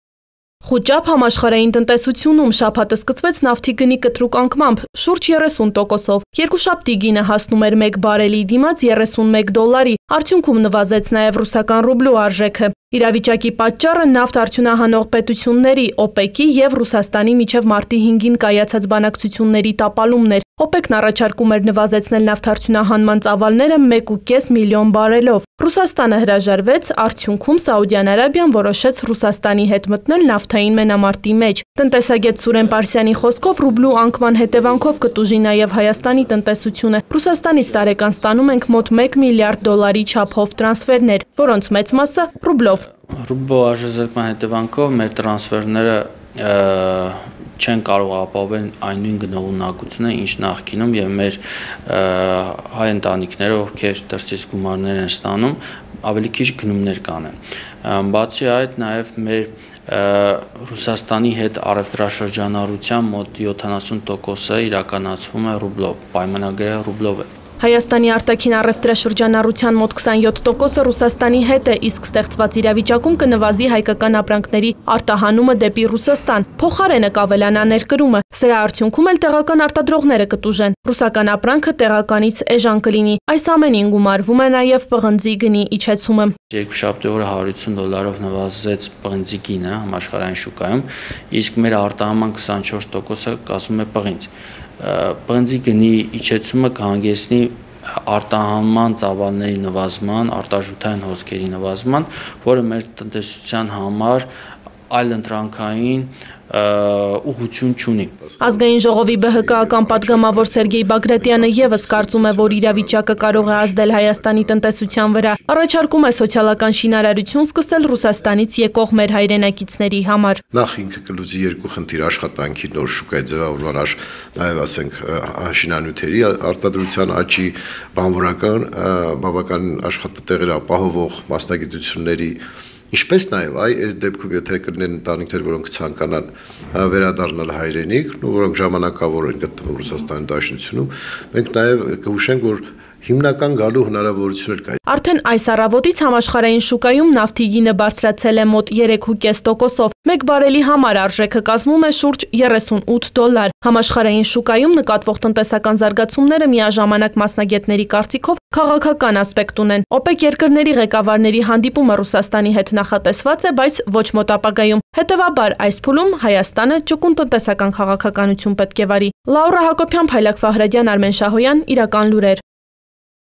Ռուսական ռուբլու արժեզրկումը կարող է ազդել Հայաստանի տնտեսության վրա:
Ռուսական ռուբլու արժեզրկումը կարող է ազդել Հայաստանի տնտեսության վրա: Մյուս կողմից էլ երկուշաբթի պղնձի գինն է նվազել 150 դոլարով: